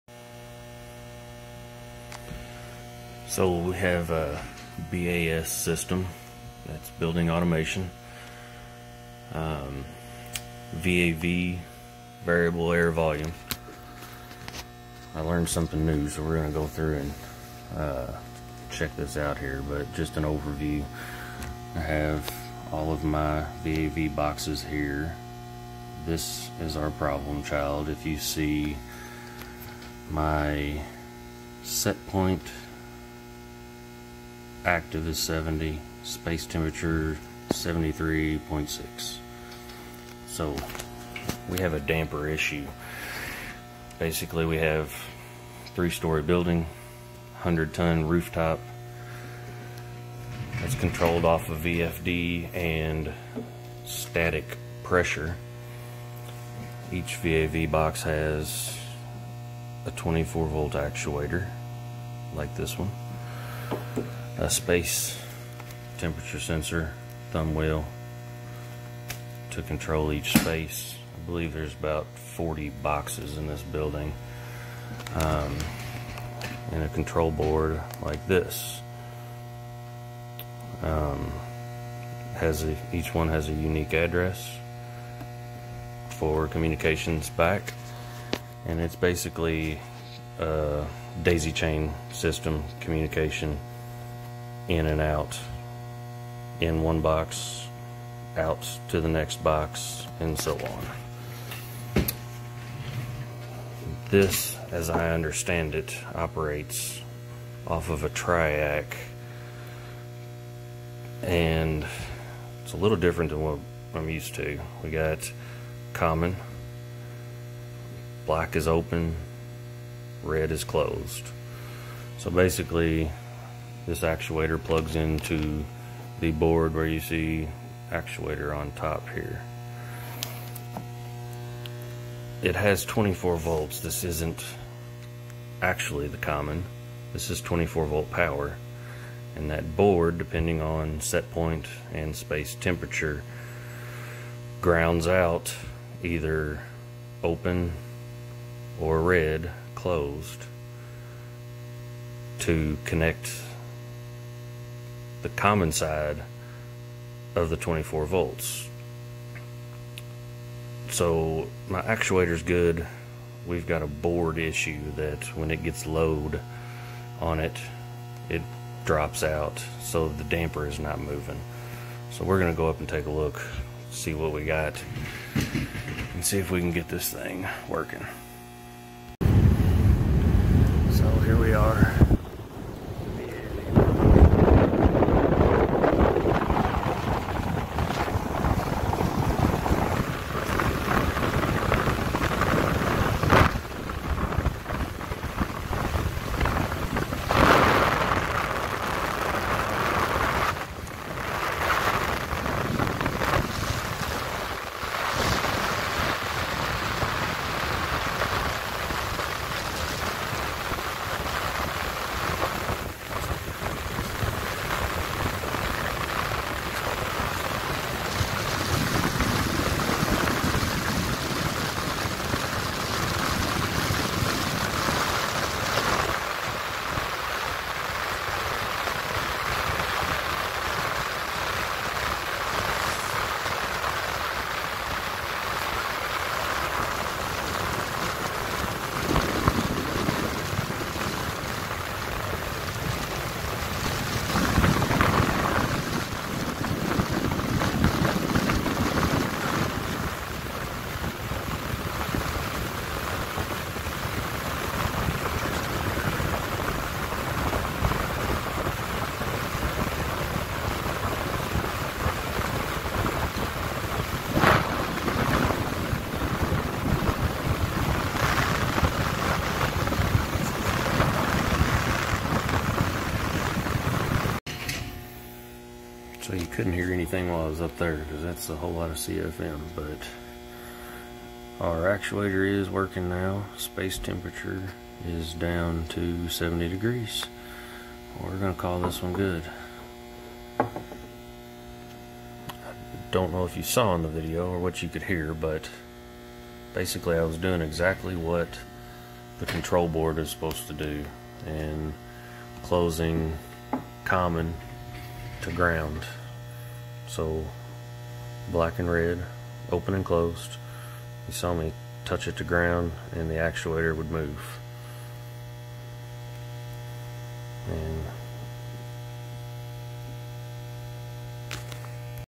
hvac service call, Old VAV system damper not working.
Building automation